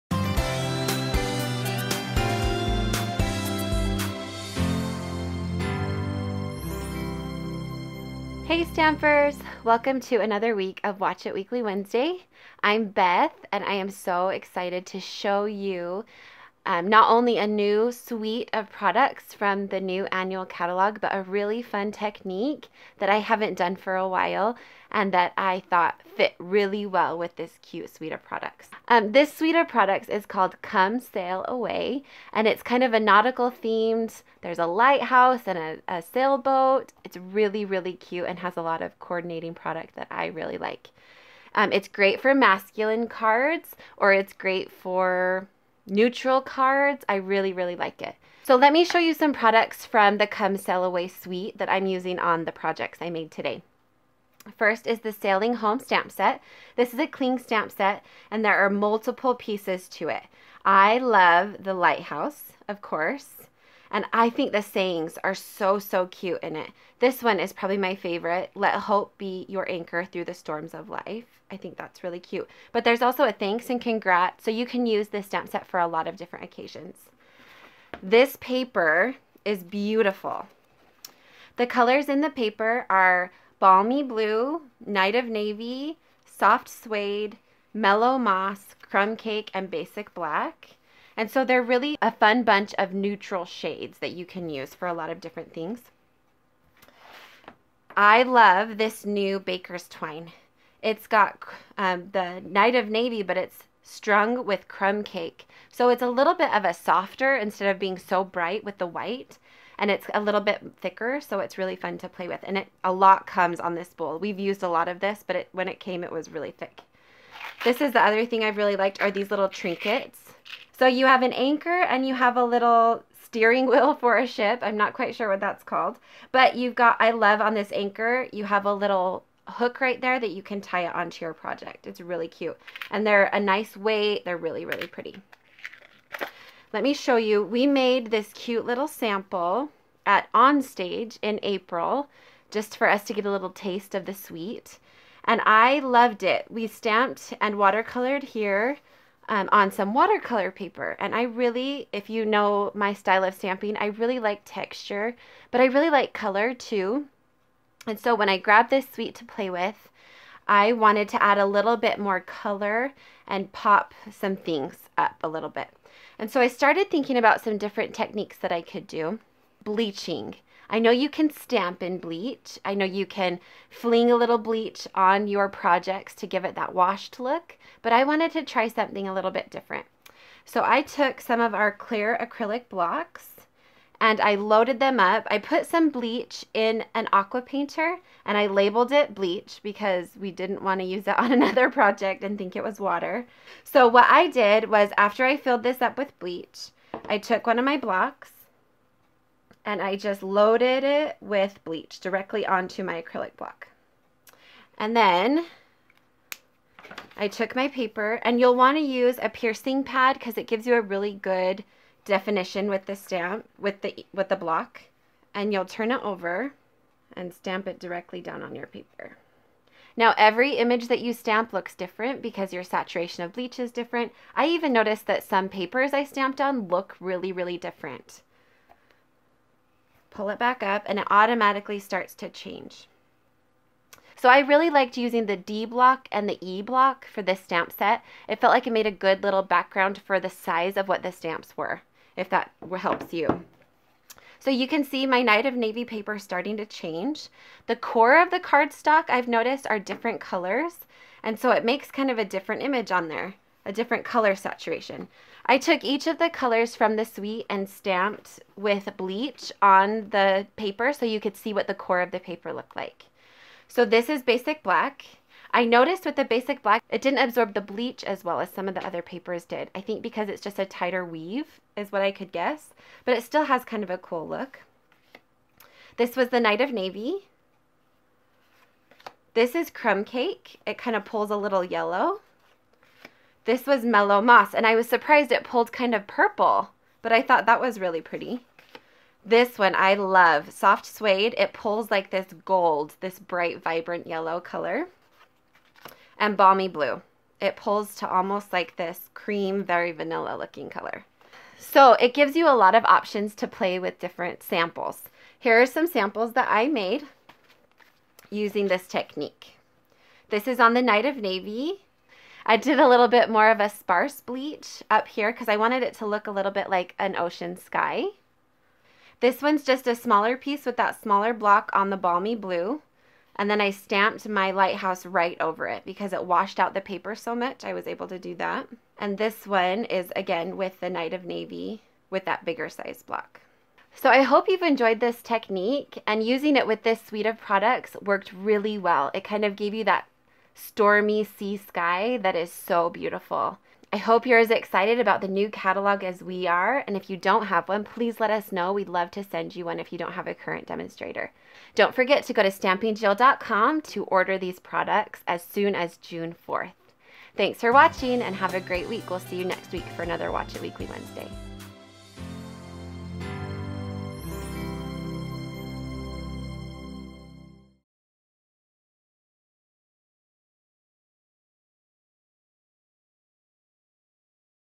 Stamping Jill - How To Use The Bleach Technique
Stampin' Up! Demonstrator Beth demonstrates an easy technique that will add so much dynamic to your cards. Bleaching! You will also see how well it goes with the Come Sail Away Suite Check it out!